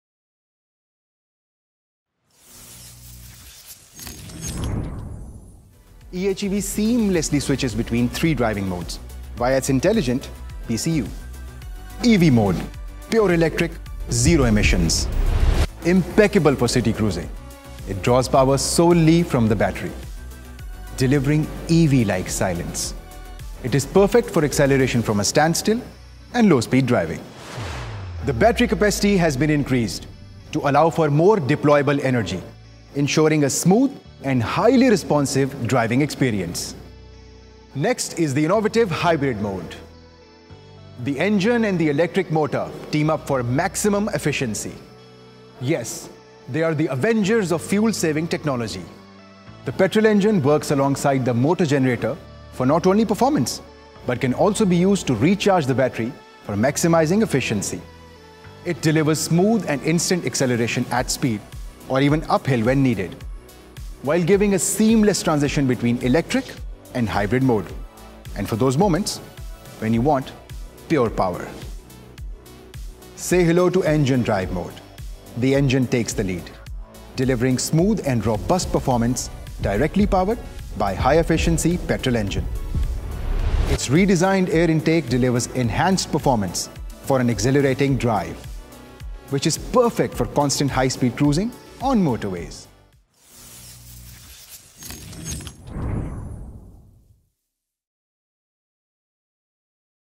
e:HEV 03 modes: EV, Hybrid, and Engine
Drive the future with Honda e:HEV – Three dynamic modes: EV, Hybrid, and Engine.